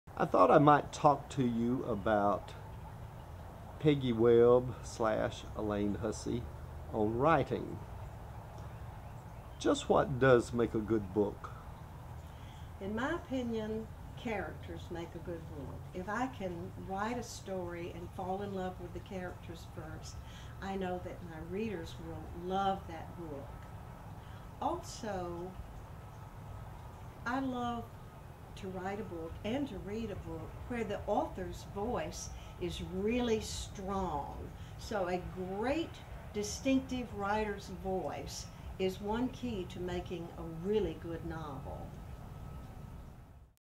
What makes a good book
Author Peggy Webb AKA Elaine Hussey offers advise for beginning writers.